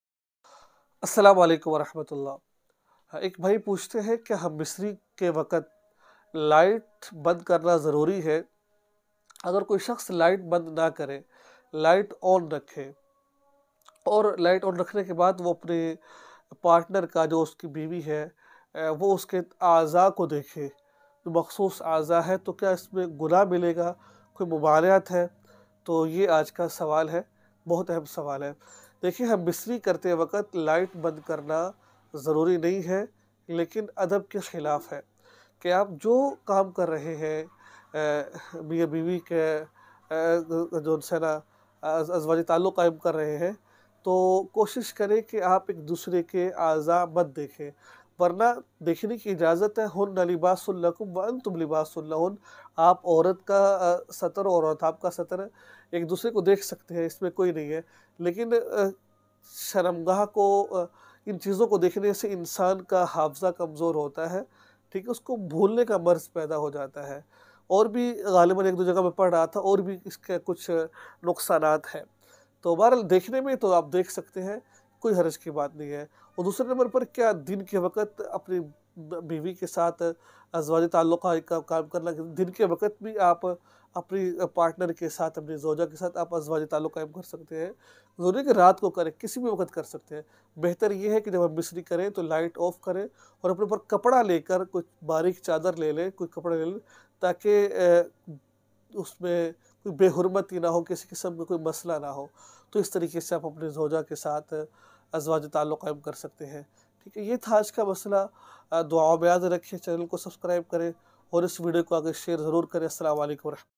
humbistari ke waqt light Band Karna Zarori Hai Kia ! Humbistari Ke Waqt Light Band Karna Zaruri Hai
Aaj Ki Video Me Hum Ap Ko Batye ge ke kya humbistari ke waqt light off karna zaroori hai Hai Kia